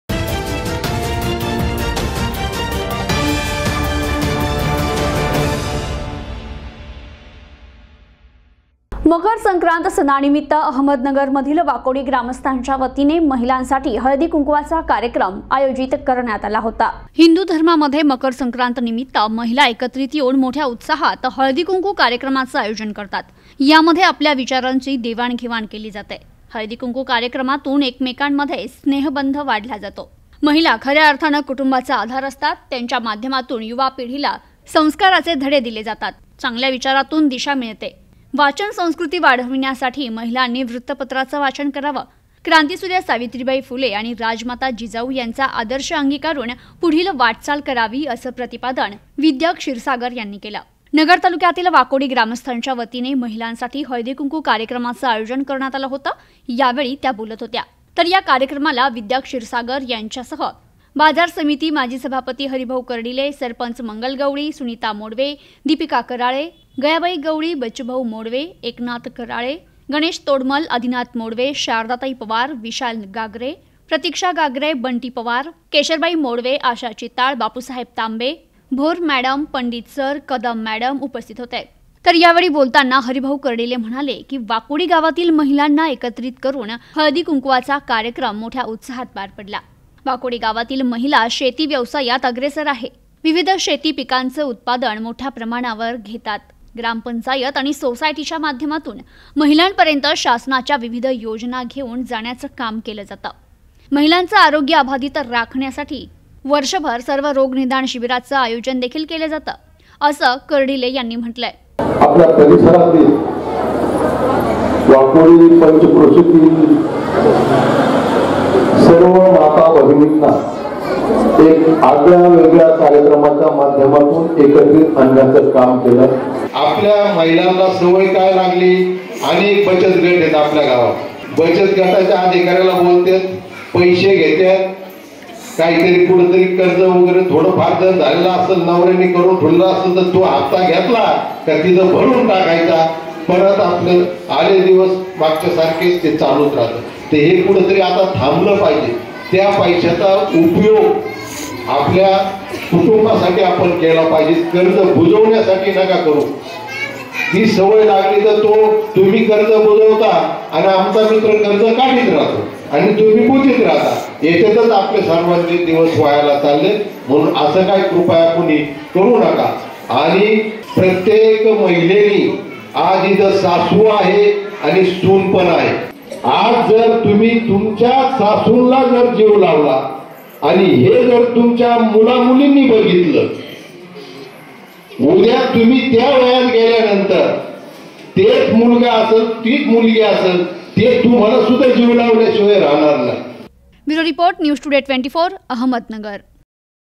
अहमदनगर : वाकोडी ग्रामस्थांच्यावतीने हळदी कुंकू कार्यक्रमाचे आयोजन
हळदी कुंकू कार्यक्रमातून स्नेहबंध निर्माण होतो - विद्याताई क्षीरसागर